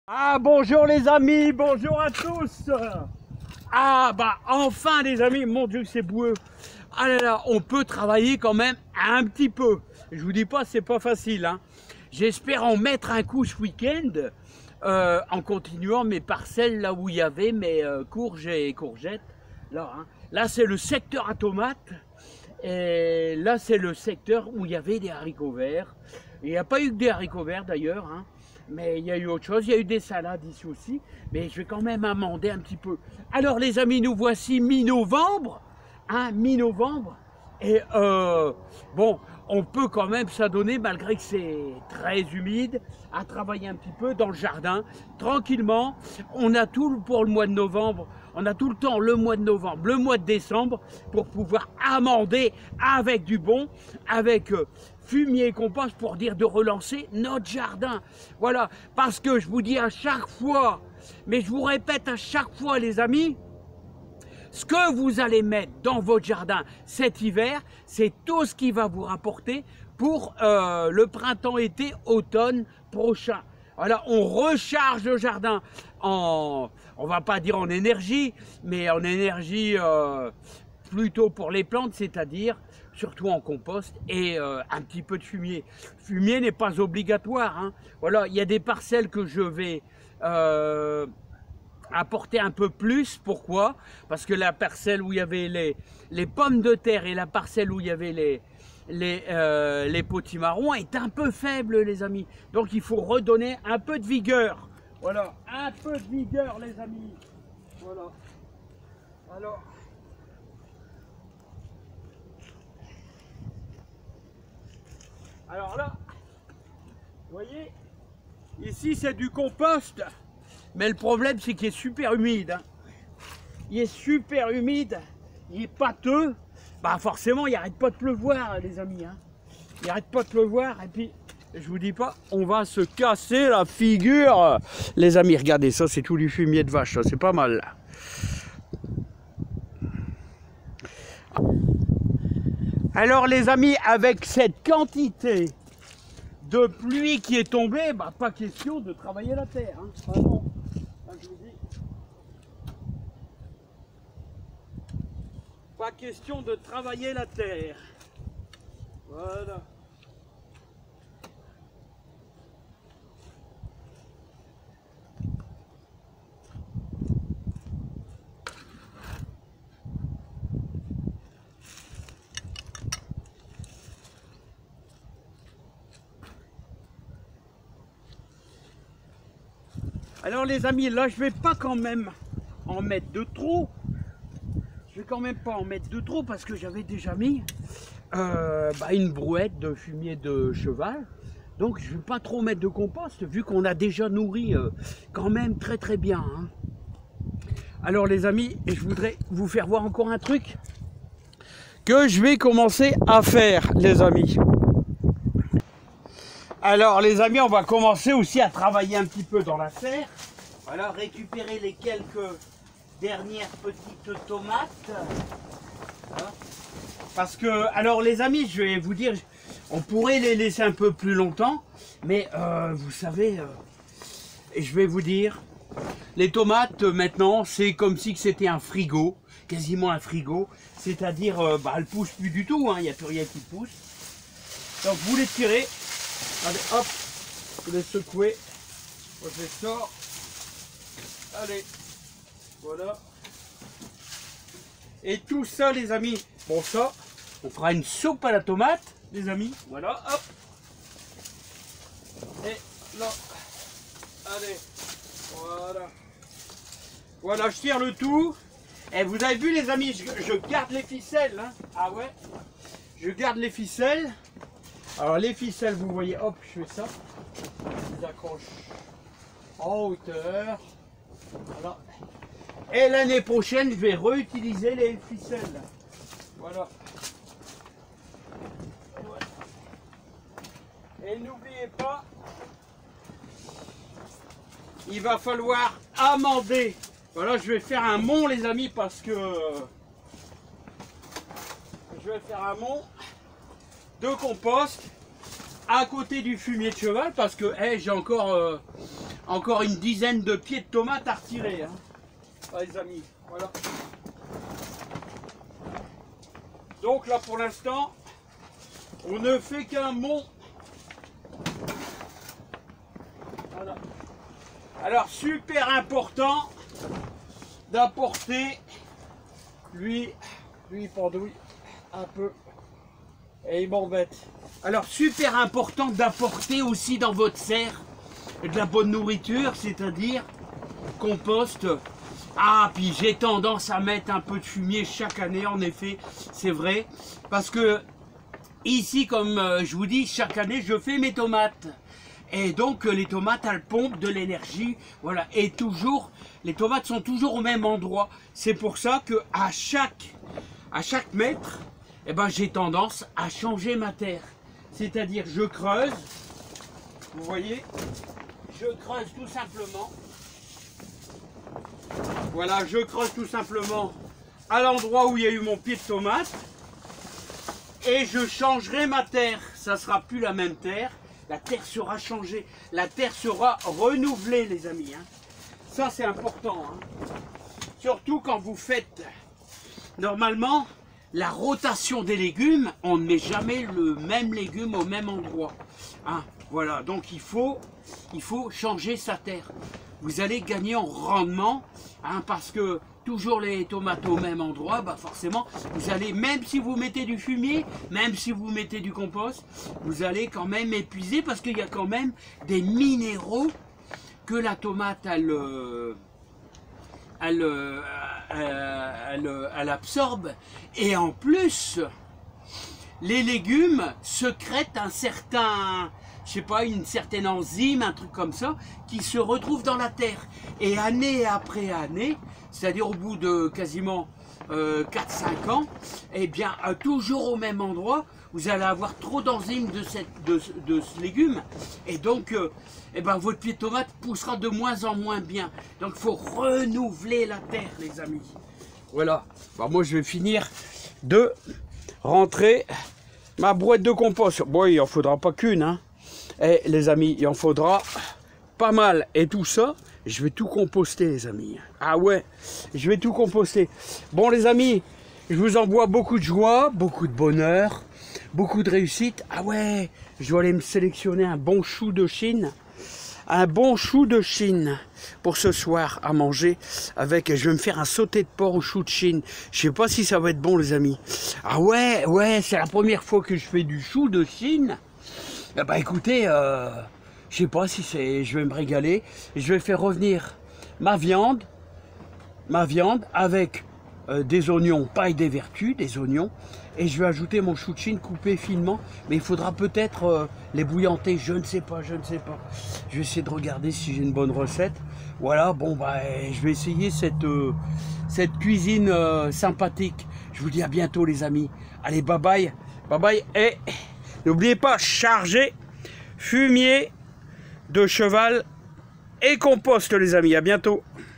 Pour réussir votre JARDIN dés le printemps PLUS JAMAIS DE PETITS LEGUMES
Nourrir votre sol est une obligation pour de gros et beaux légumes !